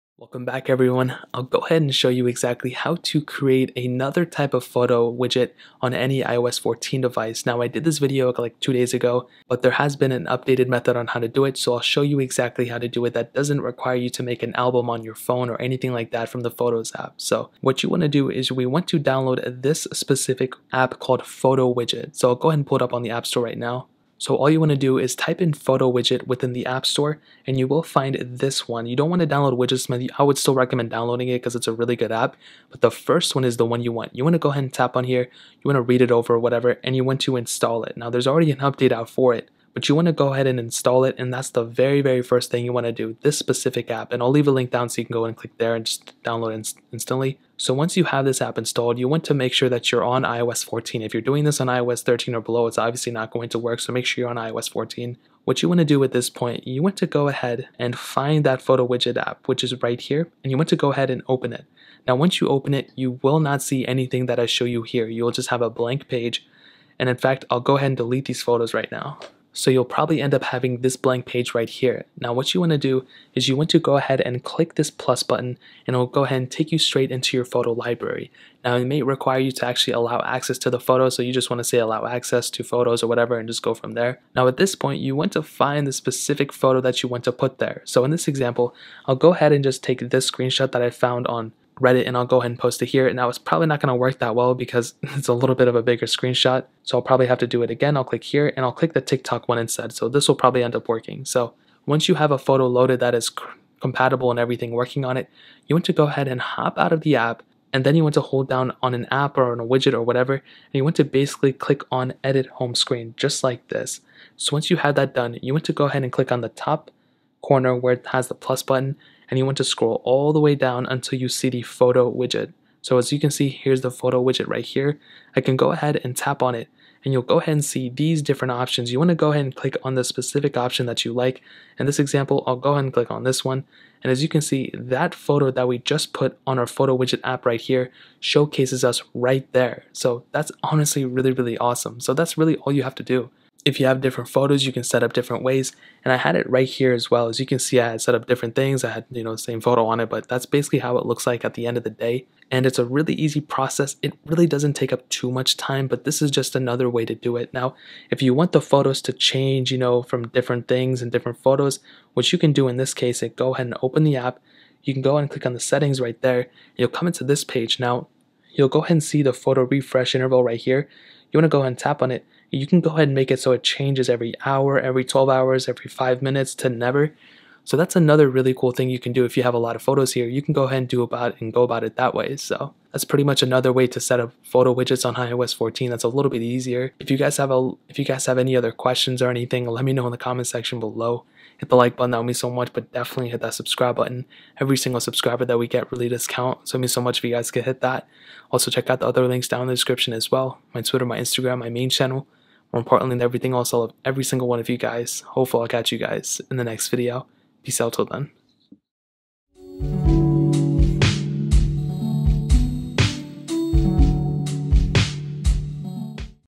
How To Set Photo As Widgets On iOS 14!
Here is exactly how to create your photo widgets on iOS 14!